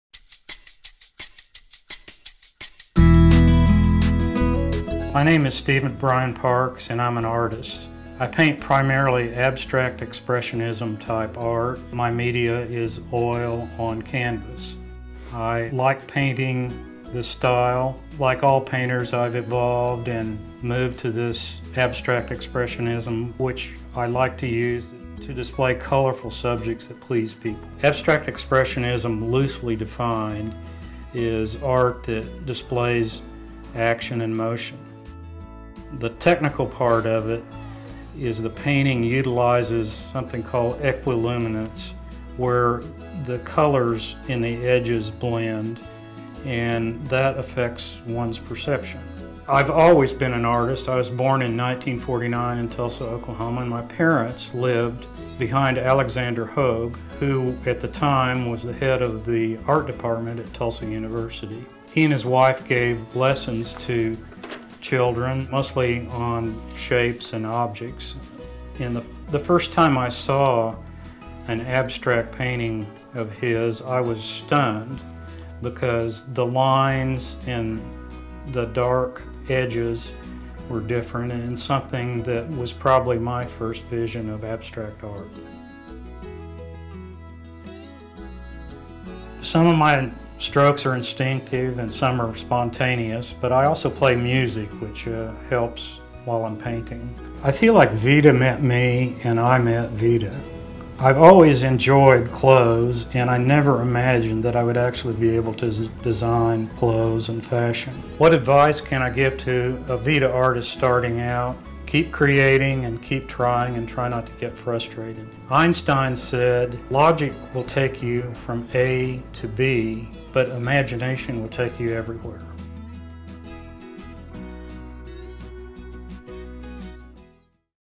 Meet VIDA Artist, Steve Parks
"Logic will get you from A to B. Imagination will take you everywhere."
Abstract Expressionist Steve Parks shares what inspired him to become a painter and words of wisdom to artists just starting out.